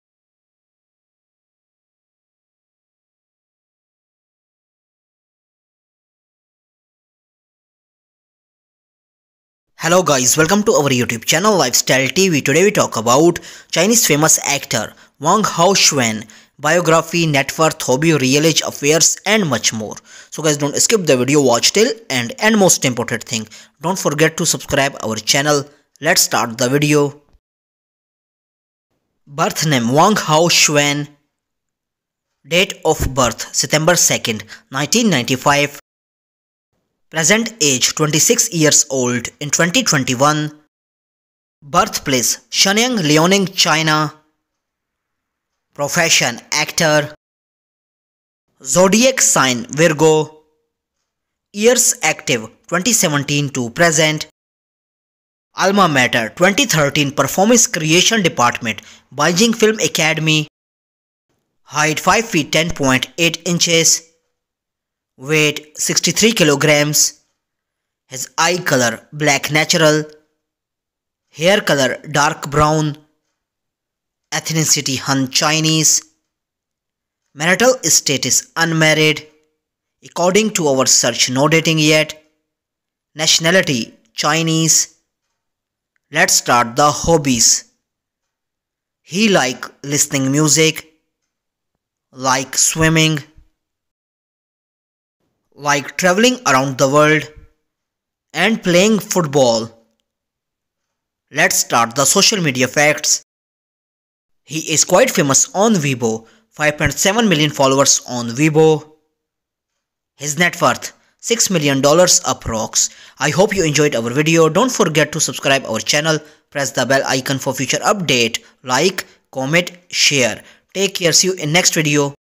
Wang Hao Xuan Lifestyle (Stick to the Script!) Biography, Age, Girlfriend, Income, Hobbies, Facts
Wang Hao Xuan Biography (Dance of the PhoenixStick to the Script!) Lifestyle, Age, Girlfriend, Income, Hobbies, Facts

Wang Hao Xuan is a Chinese actor. He graduated from the Performance Department of the Beijing Film Academy.

In 2018, he officially entered the acting circle by participating in "Youth in Your Name". In 2019, he appeared in the series "The Untamed", gaining notoriety for his role and receiving the "New Force of the Year" award at the 4th Golden Guduo Film and Television Festival.

In 2020, he was named as "100 Most Influential Faces", placing 42nd.